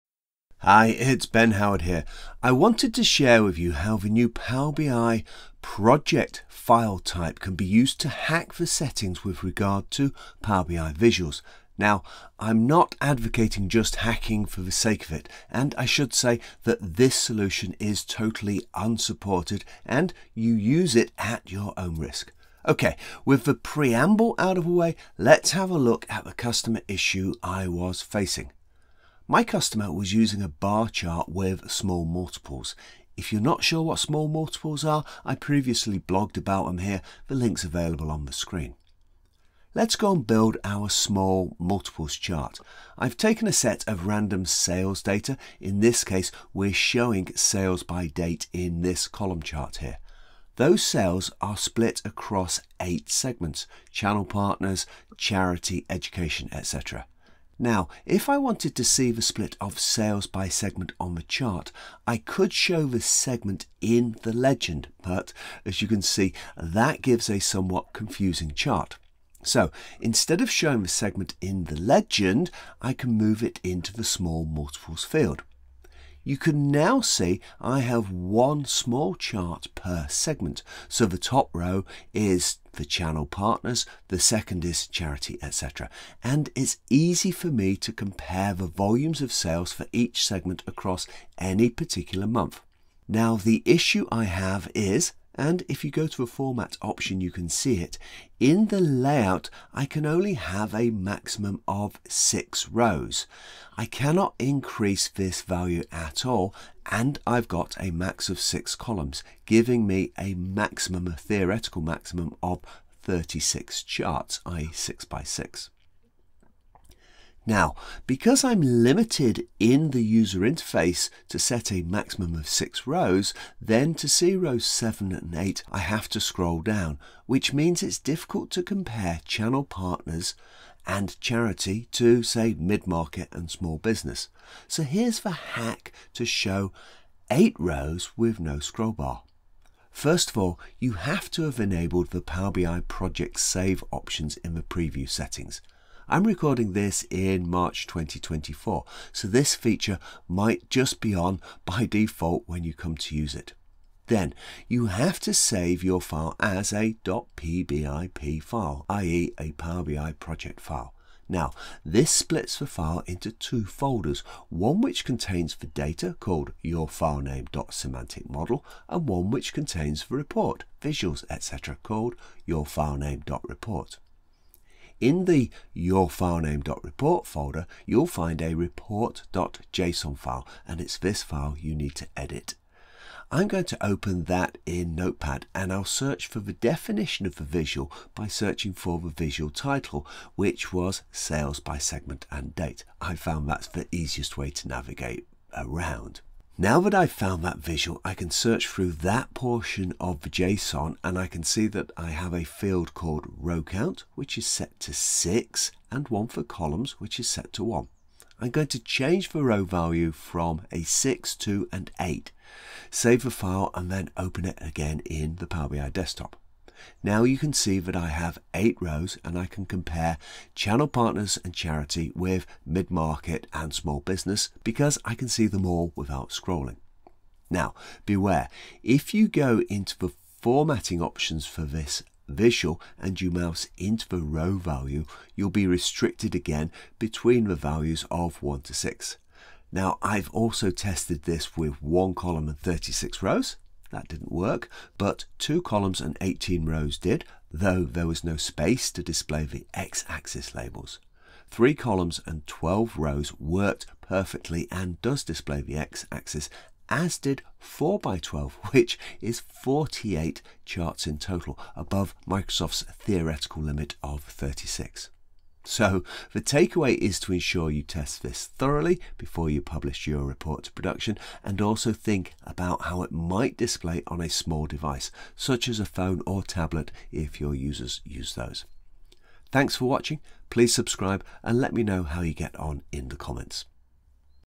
Edit (and hack) Visuals when saved to Power BI Project File type
Learn how to edit the Power BI Project File type so that you can change the properties of a visual.  This is a useful trick if the UI does not support exactly what you want it to do, for example when you need more than 6 rows or columns on a small multiples chart.